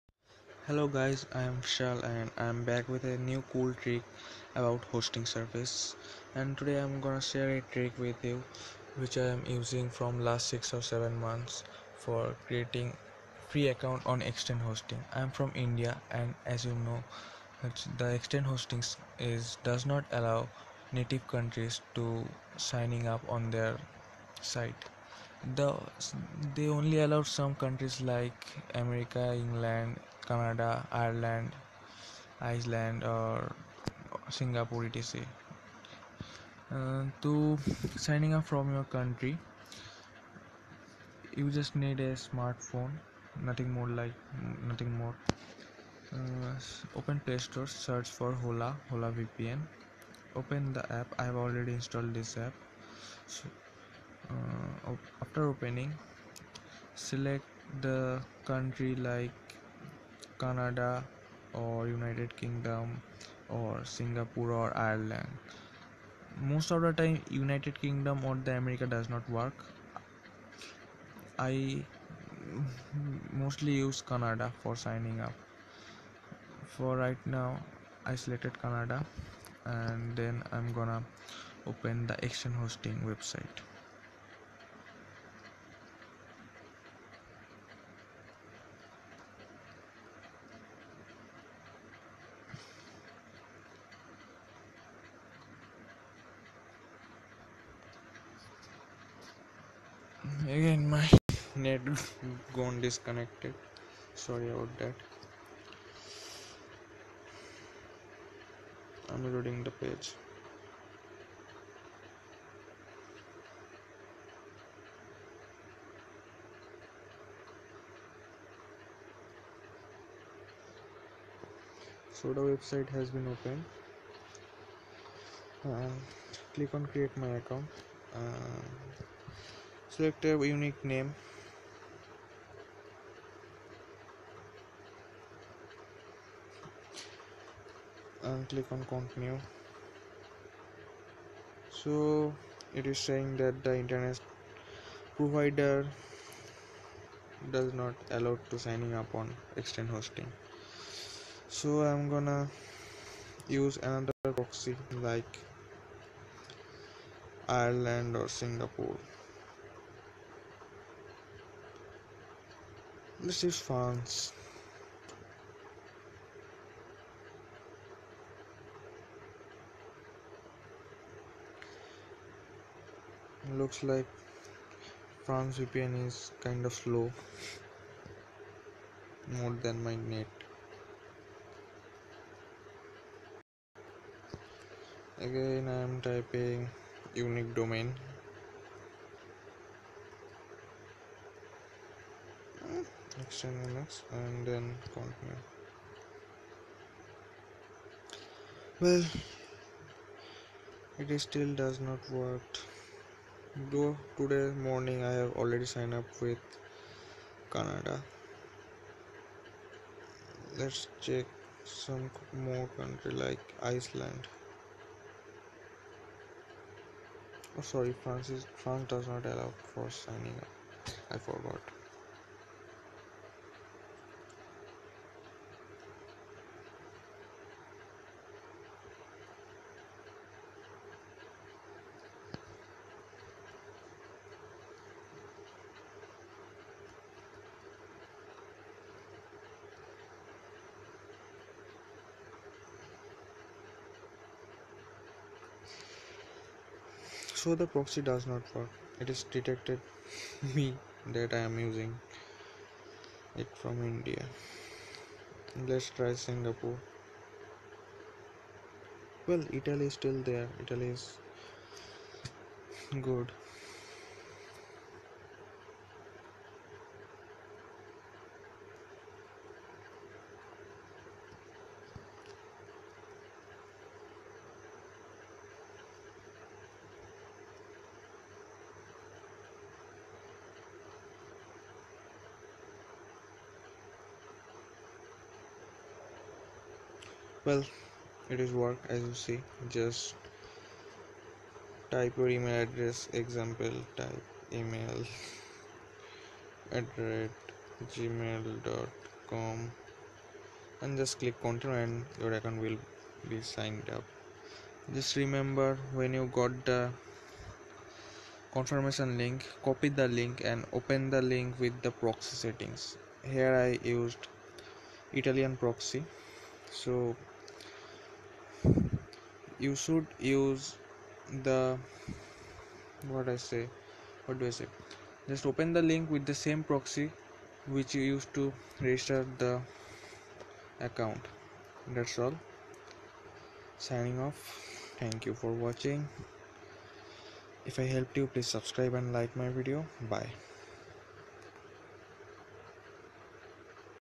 X10hosting Sign Up from not supported country from phone
Sign up on x10hosting from not supported countrys like India Bangladesh Shrianka China Pakistan Afghanistan etc. Even from mobile. No need of pc.